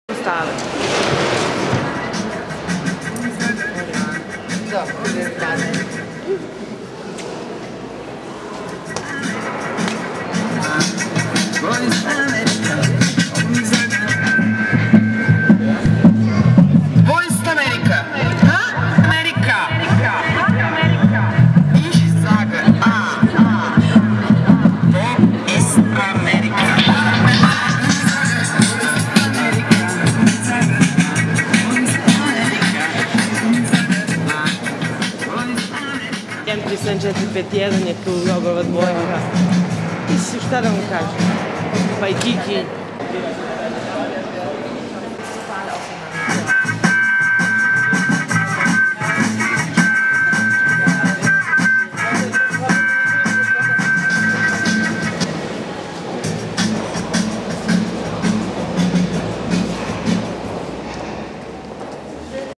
Wo ist Amerika? Videoinstallation von Biljana MIlkov, preview
Das ist ein interaktives Skulptur, ein Büroschrank der in sich ein video und ein audio kanal hat. Audio kanal ist mit einem software verbunden das durch webcamera die bewegung des Zuschauers wahrnimmt und dadurch steuert die Lautstärke von audio kanal. Man hört etwas nur wenn man sich bewegt. Wenn der Schrank zu ist herscht die Stille, auch wenn es in innerem alles aktiv ist. Es ist eine werk über regression (im Jungschem Sinn), über libido die sich in innere welt zurückgezogen hat, die in nostalgie gefluchtet hat, in einem denselben ewigen loop dessen apsurdität und unfunktionalität nur durch anderen zur Erscheinung kommt. Ich habe PureData Motion Tracking software benutzt.